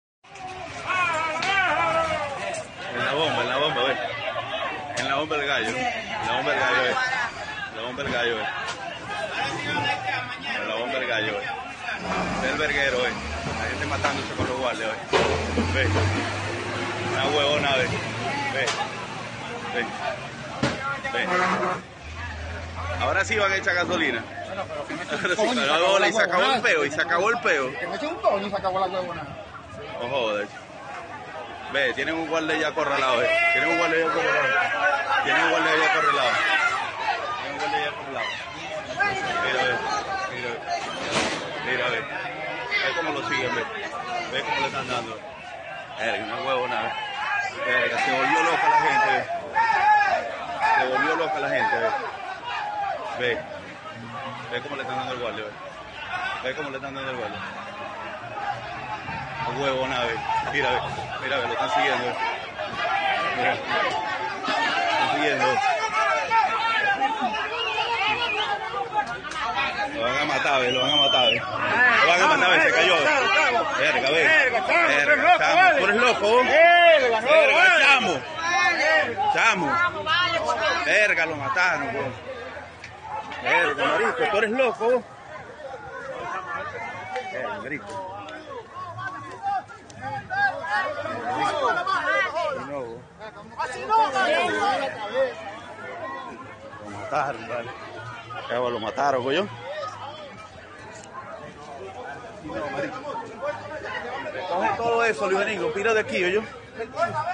Herido GNB en E/S de San Félix tras recibir un tubazo en la cabeza
Conductores de San Félix en el estado Bolívar, quienes esperaban par surtir, agredieron a un funcionario de la GNB en la estación de servicio El Gallo, el cual resultó herido tras recibir un tubazo en la cabeza. El hecho ocurrió tras armarse una trifulca.